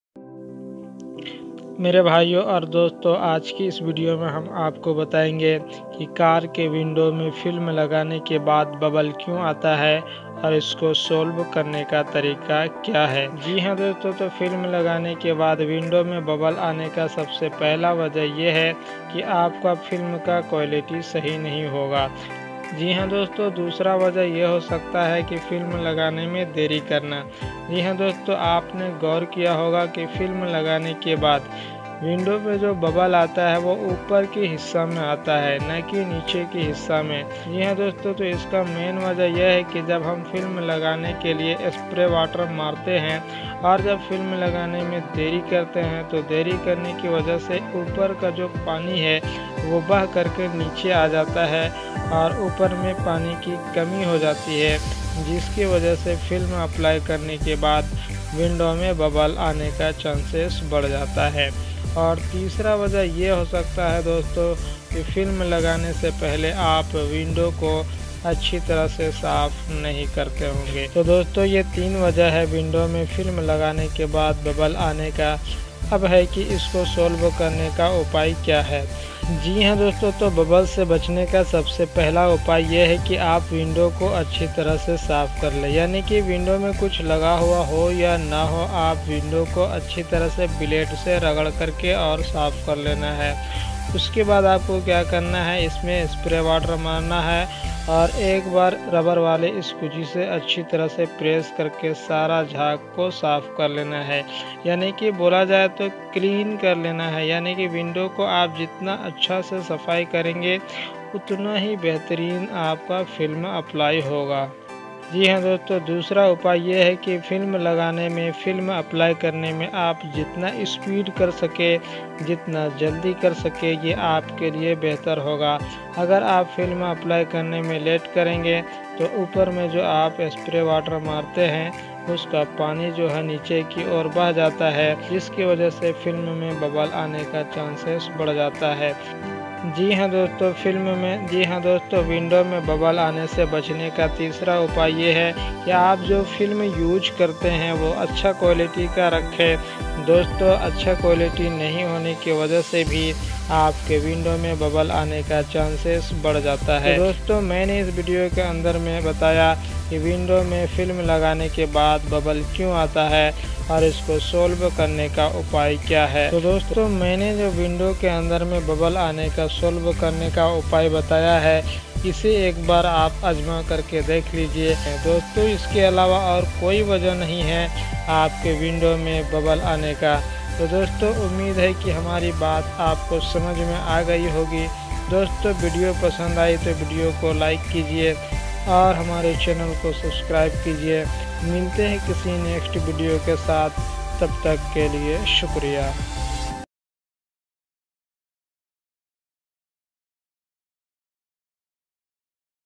Car window film problem solved
जी हाँ दोस्तो
इस वीडियो में बताया गया है कि कार के विन्डो में बबल क्यूँ आता है
धन्यवाद